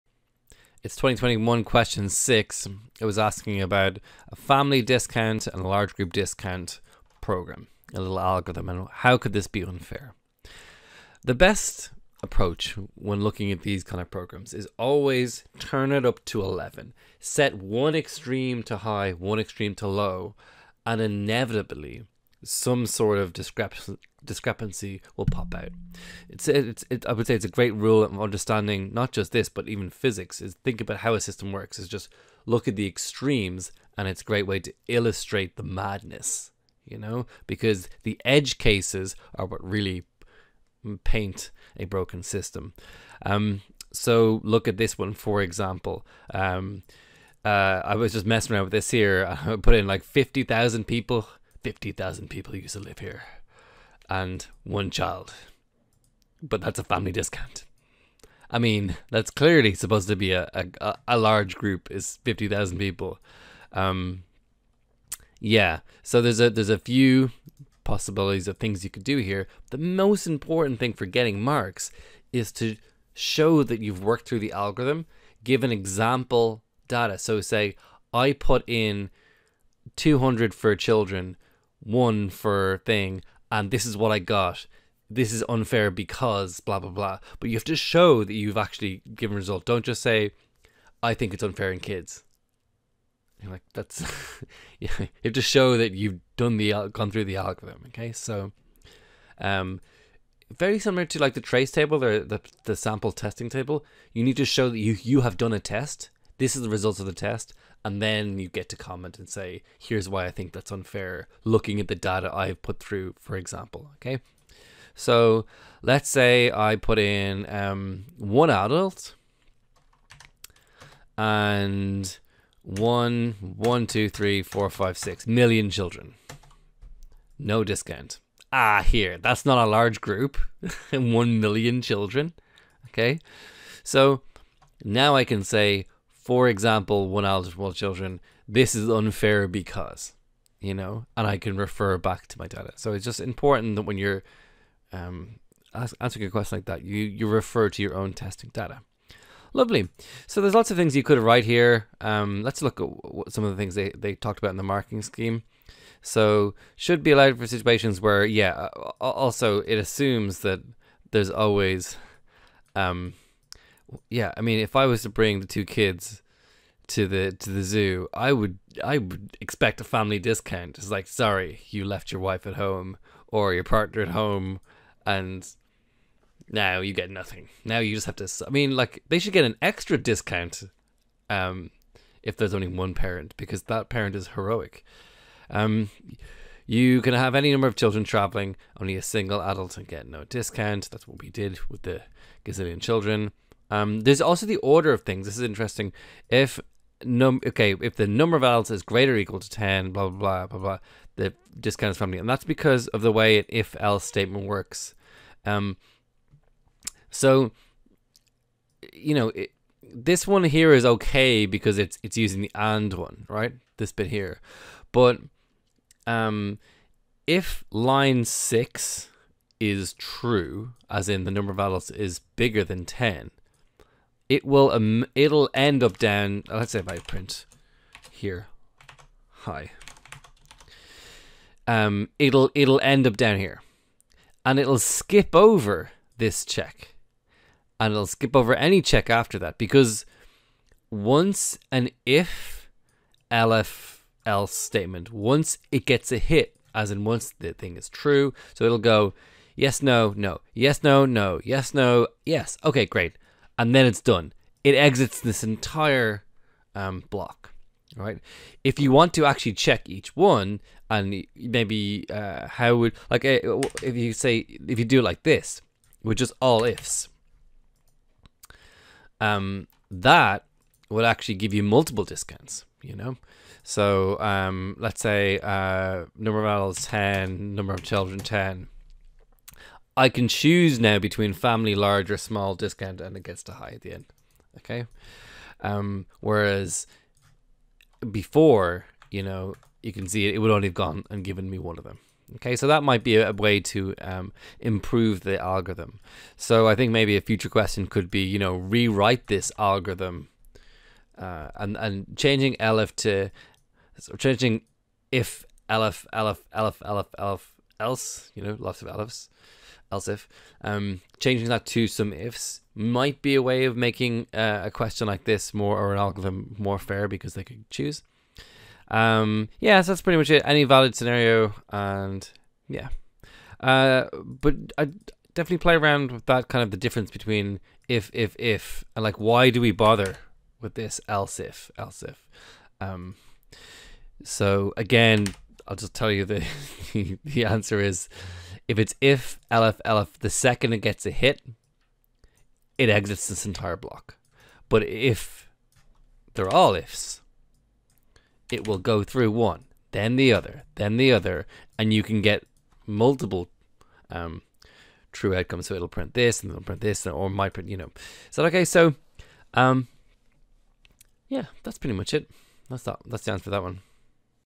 2021 HL Q6 Leaving Cert Computer Science Solution
Leaving Cert Computer Science Exam Solutions worked through and explained by Danny Murray. This one also has a lesson about a future question on binary birthday cakes.

0:00 Explanation
2:35 Marking Scheme